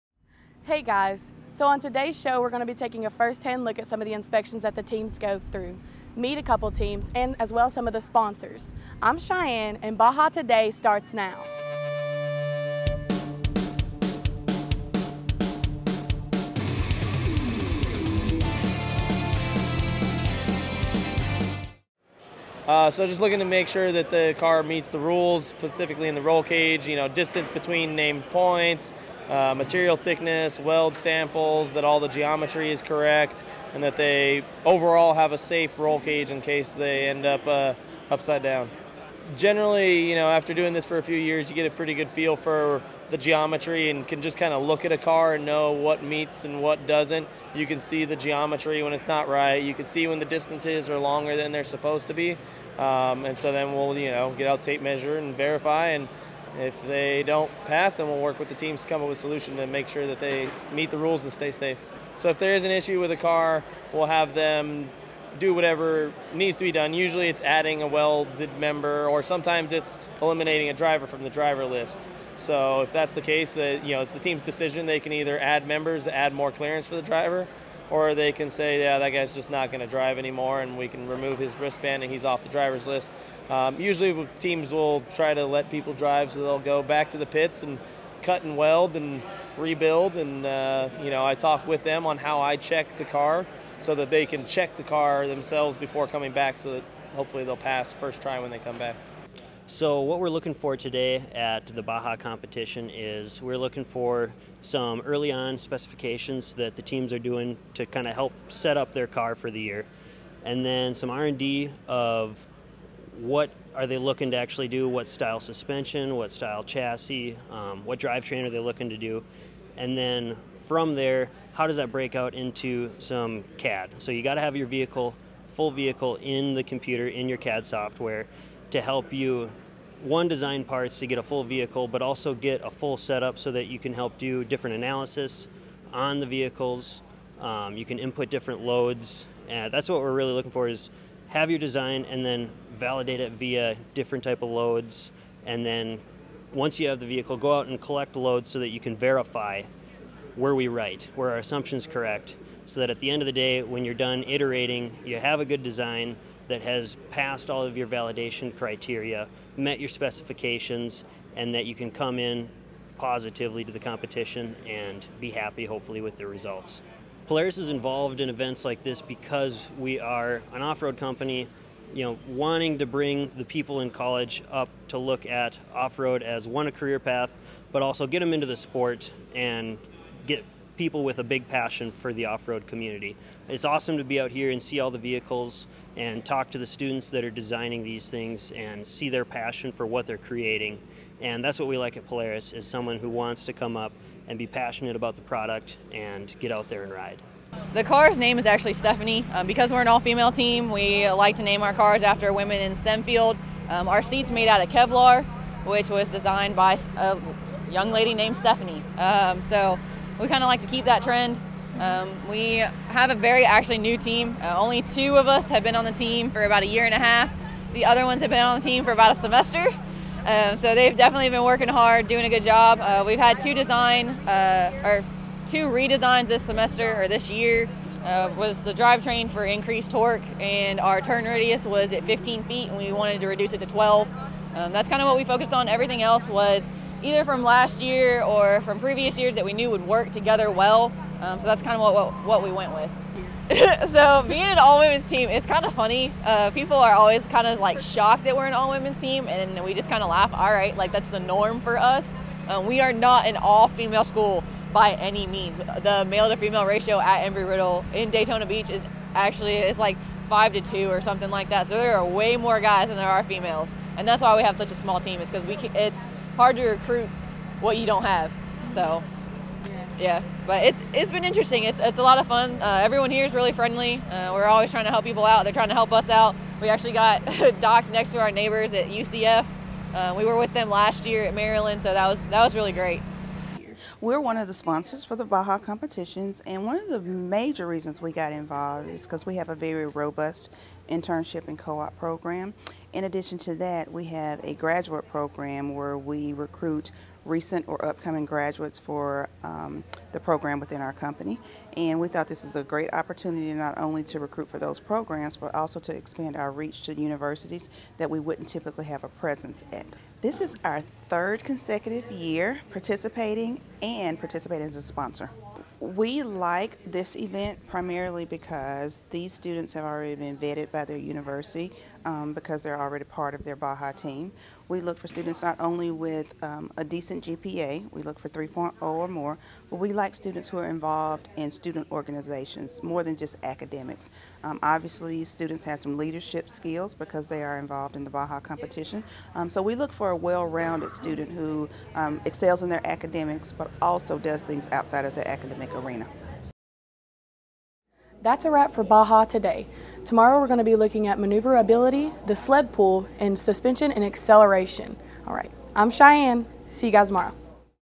Baja Today - Day 2 - Tennessee Tech April 12, 2019
Tennessee Tech hosts the Baja Society of Automotive Engineers International competition April 11-14 at Tech’s Hyder-Burks Agricultural Pavilion. This video highlights events from Day 2.